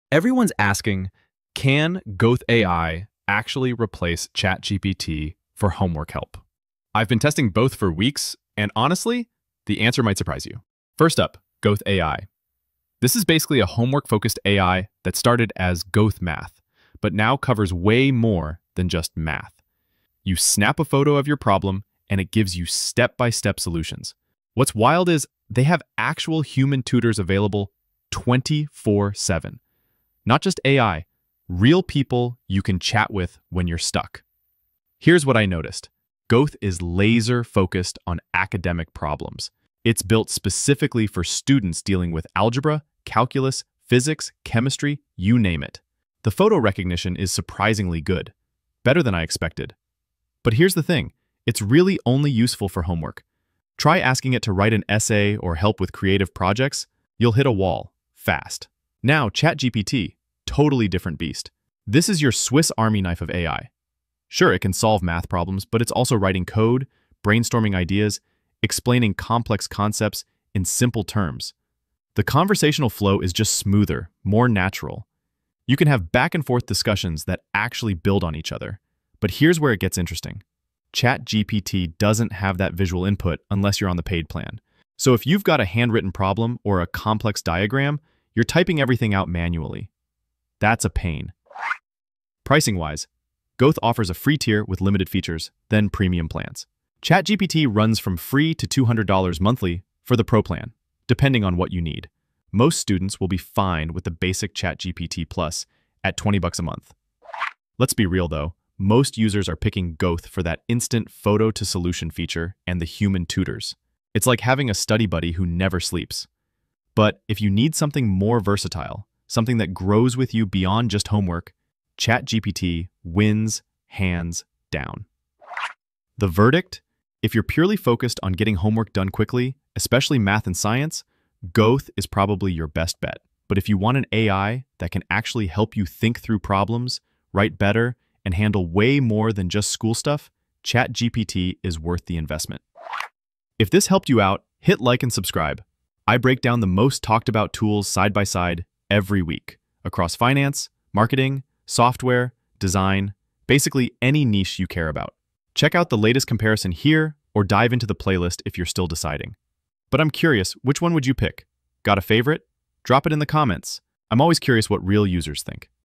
Gauth AI vs ChatGPT: Which AI Is Better for Students in 2026?
🎬 In This Video: "Struggling to choose between Gauth AI and ChatGPT for your homework and school needs?

In this video, we put Gauth AI vs ChatGPT to the test to see which tool truly delivers when it comes to solving math problems, helping with science questions, writing essays, and even offering human tutor support. You’ll learn the strengths, weaknesses, pricing, and ideal use cases for each platform — and the surprising reasons why you might prefer one over the other.

Whether you're a high school student cramming for finals or a college student juggling multiple subjects, this breakdown will help you pick the AI tool that actually fits your learning style. From instant photo-based solutions with Gauth AI to natural back-and-forth problem solving with ChatGPT, we cover it all.

👇 Check out both tools below and decide which one fits your study style best!

🛠️ Tools Mentioned:

Gauth AI